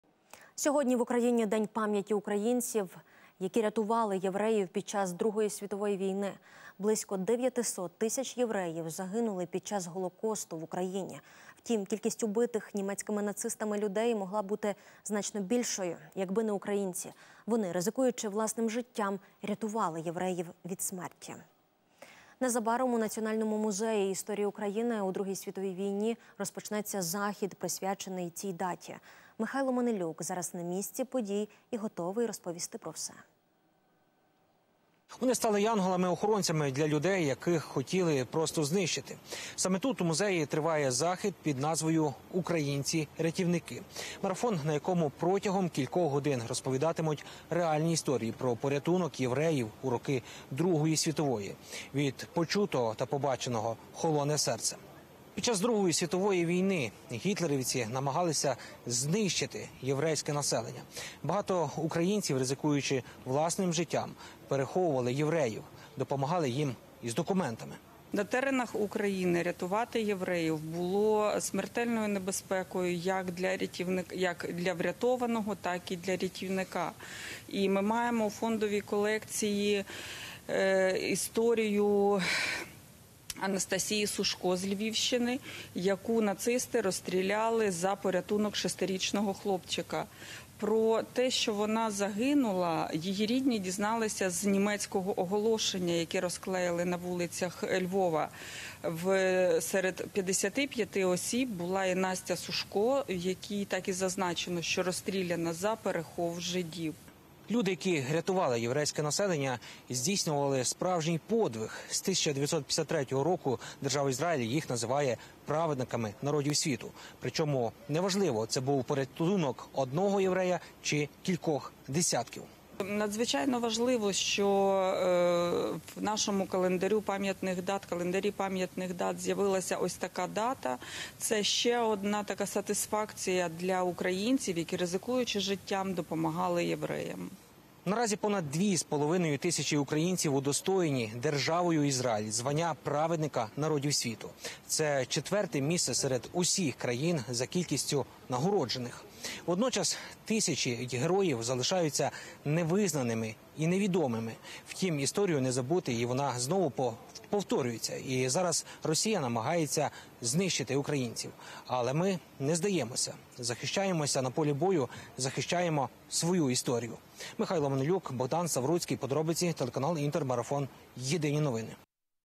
У Києві відбулася ВИСТАВКА "Українці-Рятівники. Марафон історій"
Сьогодні в Україні День пам'яті українців, які рятували євреїв під час Другої світової війни. Близько дев'ятисот тисяч євреїв загинули під час Голокосту в Україні! Втім, кількість убитих німецькими нацистами людей могла бути значно більшою, якби не українці. Вони, ризикуючи власним життям, рятували євреїв від смерті. Незабаром у Національному музеї історії України у Другій світовій війні розпочнеться захід, присвячений цій даті. Михайло Манилюк зараз на місці подій і готовий розповісти про все.